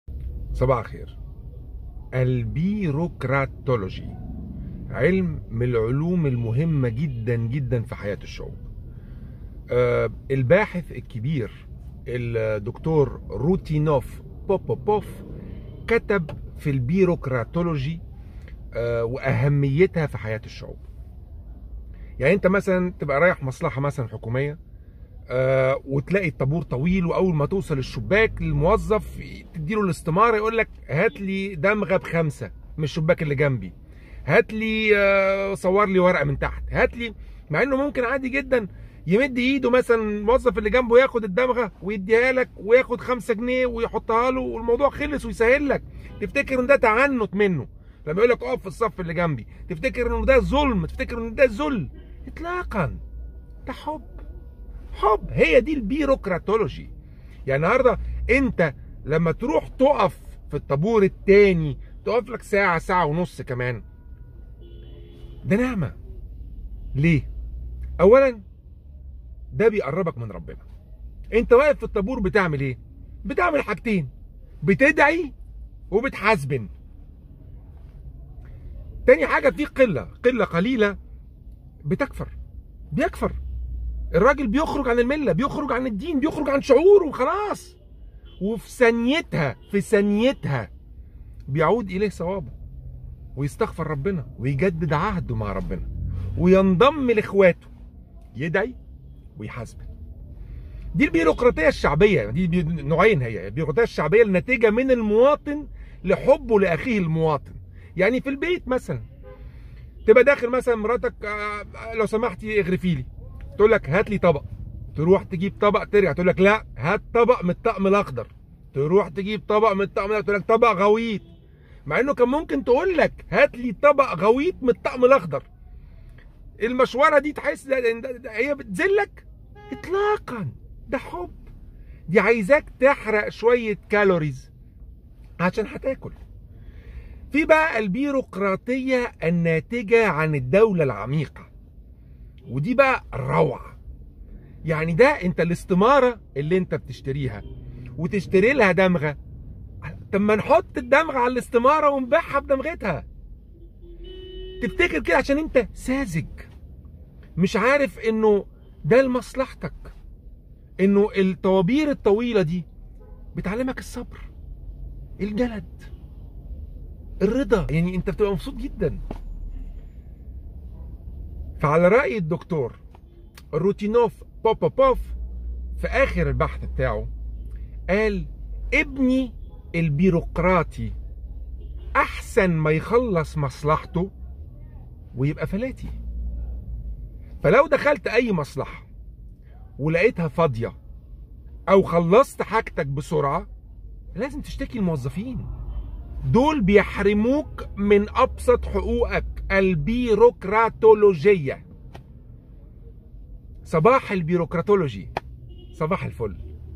بشير شوشةI البيروقراطية...بكل حب ❤
البيروقراطية و فوائدها للمواطن
لماذا نكره البيروقراطية و هي لمصلحة الشعوب